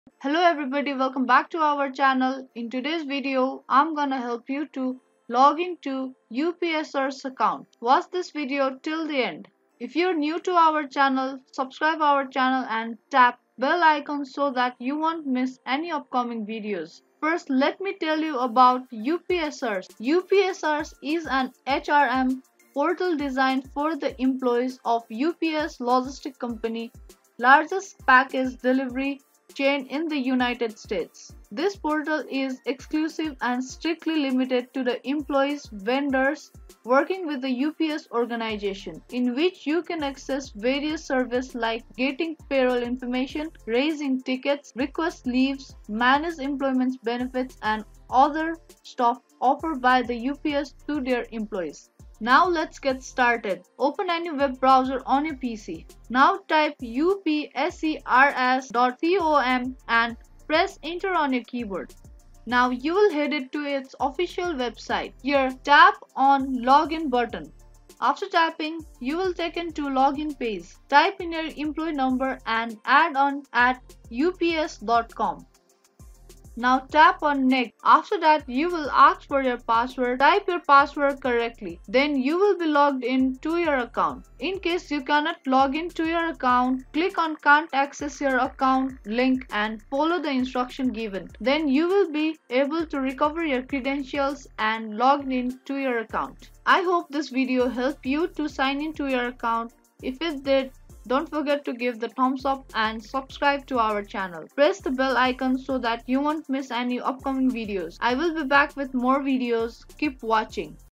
UPS Employee Login | How to Login Upsers.com Account (2023)
Learn how to login upsers employee portal account and how to login upsers.com.
This quick video will guide you to sign in to upsers employee portal account.